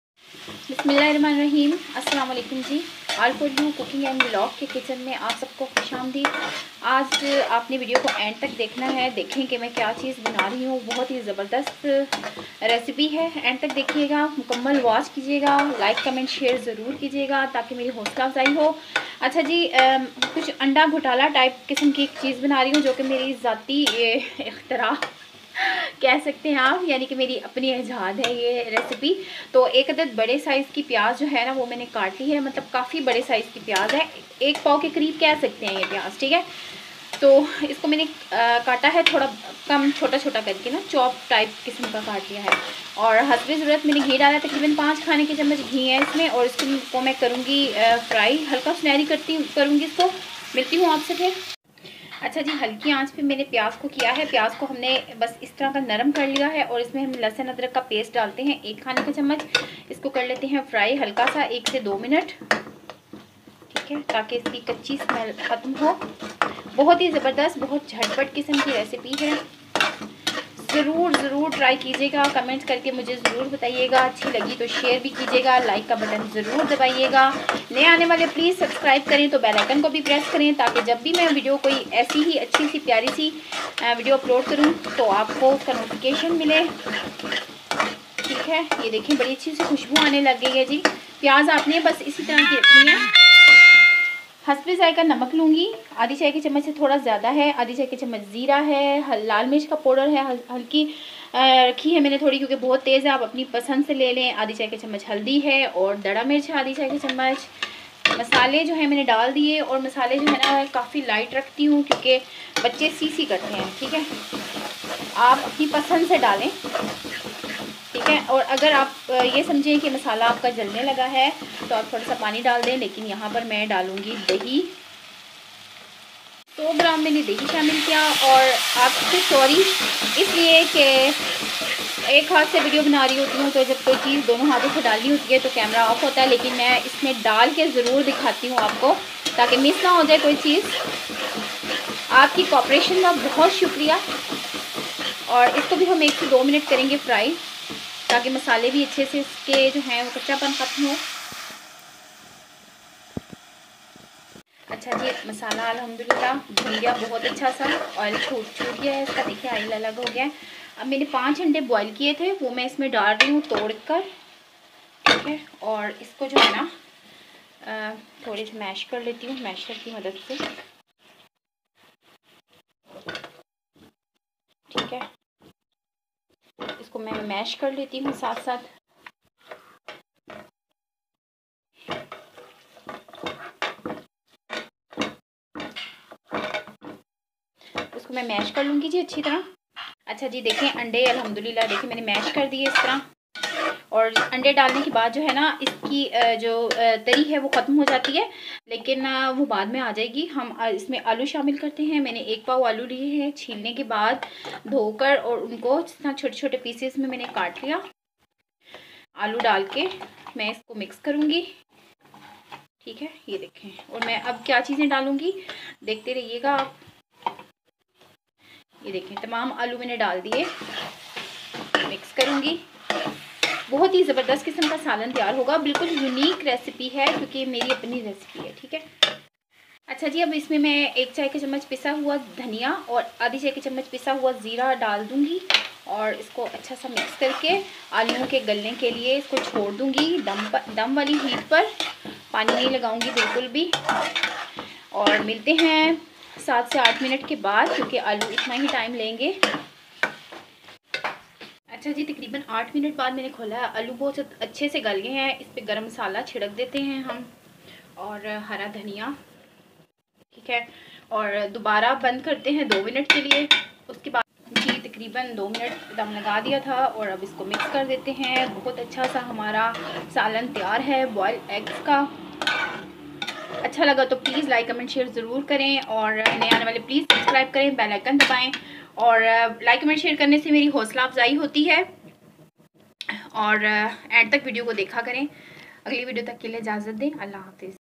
Ubley huy andon ka salan | boiled eggs curry | all for you cooking and vlog
aoa
This is my new recipe about boiled egg Curry. how to boiled egg gravy .uble huy andon ka salan.this is very easy and delicious food recipe .good for health. I hope you like this video and share.
Uble andon ka salan
how to boiled eggs curry
boiled egg gravy